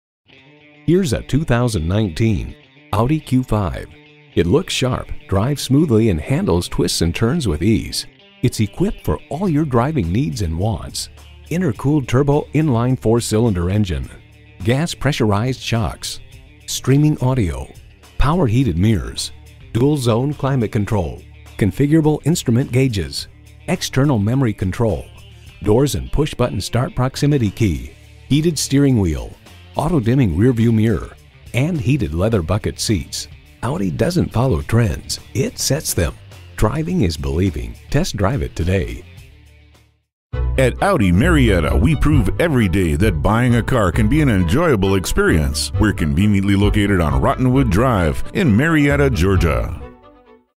Used 2019 Audi Q5 Marietta Atlanta, GA LR35508 - SOLD
Year: 2019
Make: Audi
Model: Q5
Trim: 2.0T Premium Plus
Engine: 2.0L TFSI
Transmission: 7-Speed Automatic S tronic
Color: Red
Interior: Atlas Beige
Mileage: 7447
Stock #: LR35508
VIN: WA1BNAFYXK2016754

Address:
1715 Cobb Parkway South